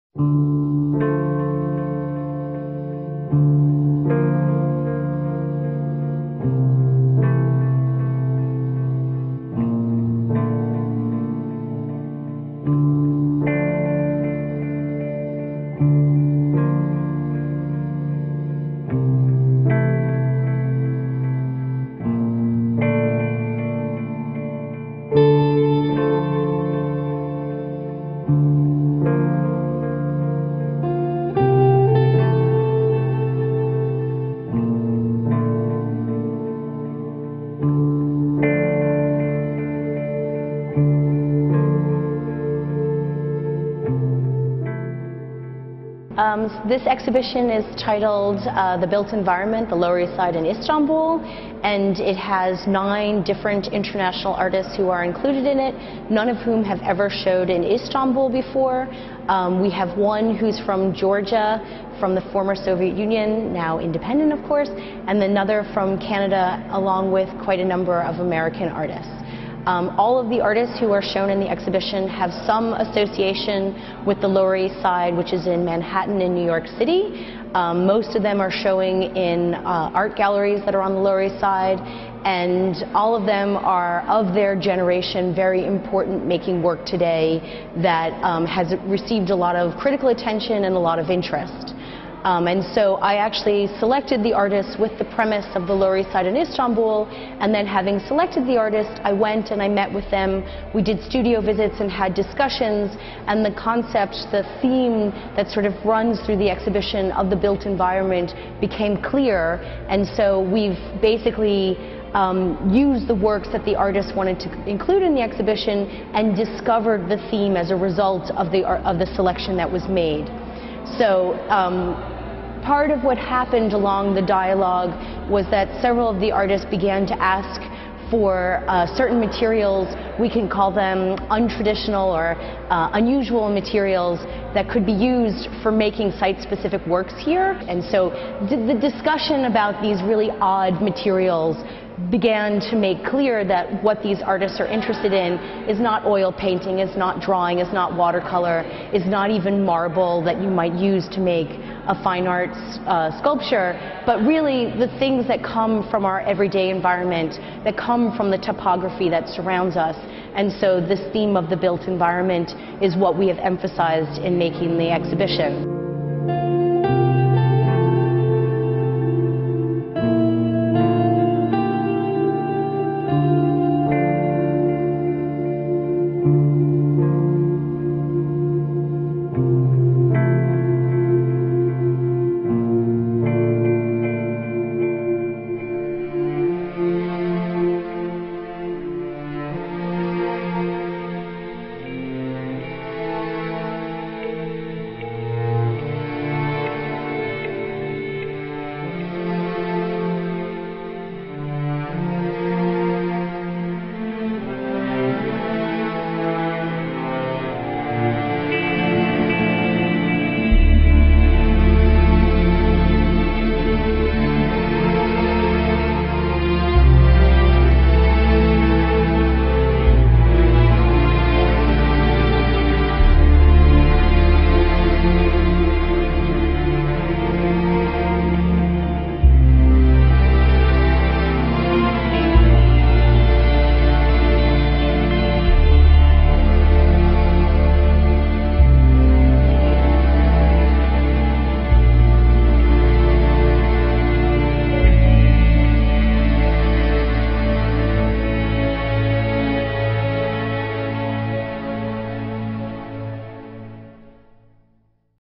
The Built Environment | Lower East Side İstanbul'da | Mixer | ARTtv
The Built Environment-Lower East Side İstanbul'da
19 Eylül - 2 Kasım 2014
Mixer

Mixer yeni sezonda işleri İstanbul’da daha önce gösterilmemiş olan New York Lower East Side'dan dokuz sanatçıyı bir araya getiren The Built Environment, Lower East Side İstanbul’da projesini sunmaktan mutluluk duyuyor. Fotoğraf, resim, kolaj, baskı, heykel ve video gibi tekniklerle işlerini üreten bu sanatçıların bazıları, İstanbul’un sosyal ve kentsel özelliklerine göndermeler yapan mekana özgü işler ortaya koyuyor. Mixer’in Tophane’deki galeri mekanında, 19 Eylül – 02 Kasım tarihleri arasında izleyiciyle buluşacak olan The Built Environment, Lower East Side İstanbul’da inşa edilmiş alanları kentsel topografya bakımından ele alarak, çevremizin düzen ve yapısının hayatlarımızda nasıl bir kaybolmuşluk, yüzleşme, ve hatta anksiyeteye yol açabildiğini irdeliyor.
The Built Environment, Lower East Side İstanbul’da günlük yaşamlarımızın ve sanatçıların üretimlerinin etrafımızı saran çevre ve bu çevreyi yönlendiren bilgi sistemleri, yapılar ve çevresel kontrol tarafından ne şekilde sınırlandırıldığını inceliyor. Sergiye katılan sanatçılar, günümüz çevresinin mimari biçim ve sanatsal obje düşünceleriyle nasıl kesiştiğini ve bu kesişim noktalarının sosyal manipülasyon, organizasyon ve bilginin yayılmasına ilişkin sistemlerle olan ilişkisini sorguluyor. Sanatçılar zaman zaman kaideleri bozmayı başararak etrafımızda olan biteni gözler önüne seriyor. Hem sakini oldukları hem de çalıştıkları New York Manhattan’daki Lower East Side (LES) bölgesini ufuk açıcı projeleri için bir platform olarak kullanan bu sanatçılar ve The Built Environment, Lower East Side İstanbul’da projesi LES’i mesken edinmiş yaratıcı oluşumların yaklaşım ve enerjisini İstanbul Tophane’ye taşıyor.
Projenin küratörlüğünü, kendisi de New York’ta yaşayan ve Sotheby’s Institute of Art ile Columbia University Barnard College’da eğitim veren ve Performa 13’ün 2015’te yayınlanacak olan kitabının editörlüğünü yapan sanat tarihçi, yazar ve küratör Kathleen Madden üstleniyor.
Sanatçılar Robin Cameron, Anna K.E., Carol Szymanski, Josh Tonsfeldt ve küratör Kathleen Madden 2014 Turkish Cultural Foundation Cultural Exchange Fellowship tarafından kısmen desteklenmektedir. Bu proje, The American Turkish Society'nin Moon and Stars Project fonu tarafından desteklenmektedir.